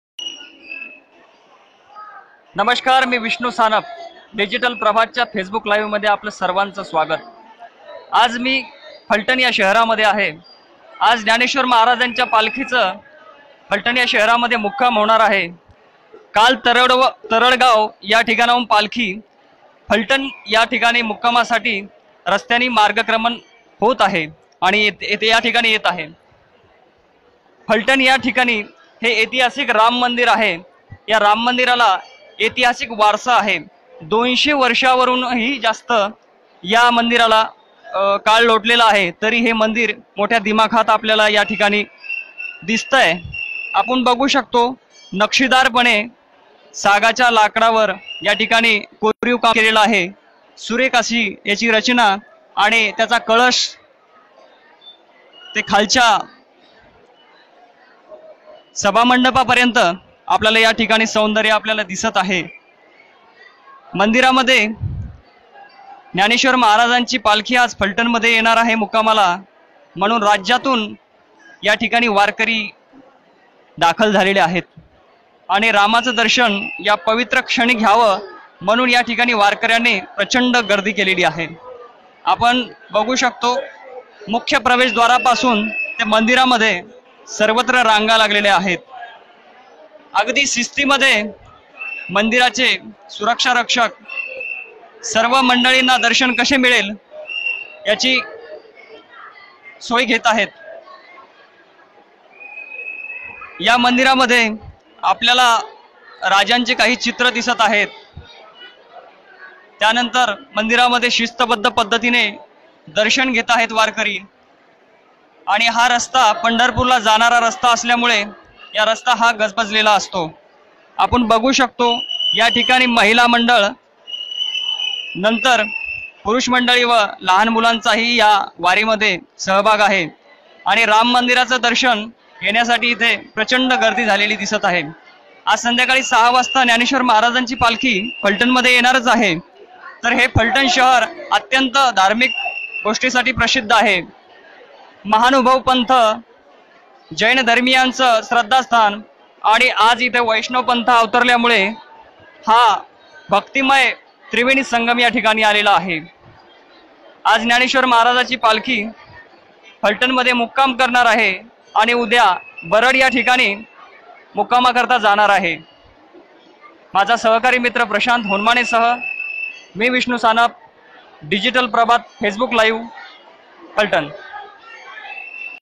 फलटण मध्ये पालखीच्या आगमनासाठी वारकरी झाले आतुर... | Prabhat Online News
संत ज्ञानेश्वर महाराज पालखीचे आज सहा वाजता फलटण शहरात आगमन होणार असल्याने शहरात आनंदाचे वातावरण असून शहरातील पुरातन काळातील ऐतिहासिक राम मंदिरात वारकऱ्यांनी दर्शनासाठी गर्दी केली .